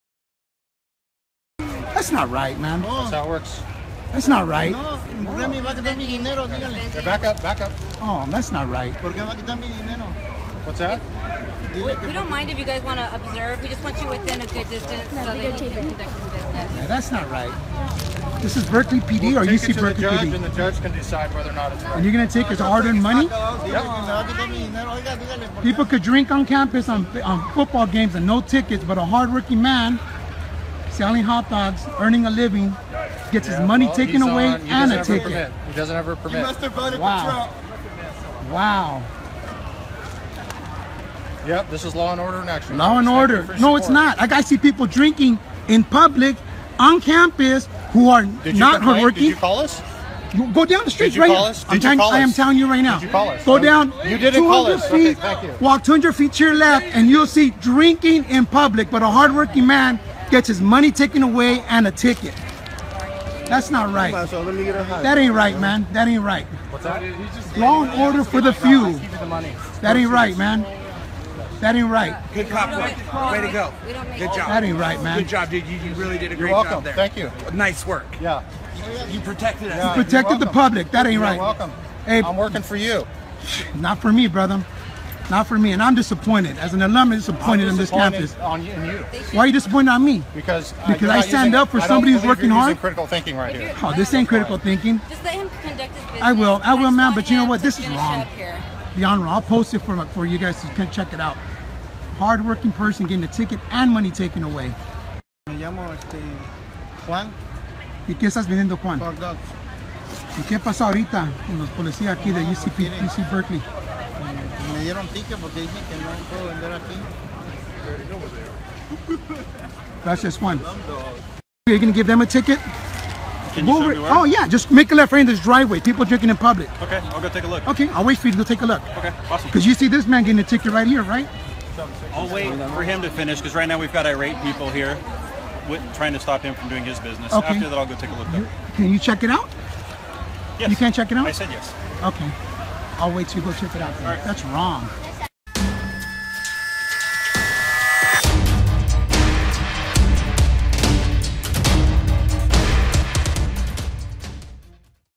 Policía de California multa y quita su dinero a vendedor hispano
Fecha: 12/09/2017
Hora: 03:58
Lugar: California, EU
Tema: Migrantes
Un oficial de policía de la Universidad de Berkeley le decomisó el dinero y multó a un vendedor ambulante que vendía perros calientes frente al estadio de la universidad.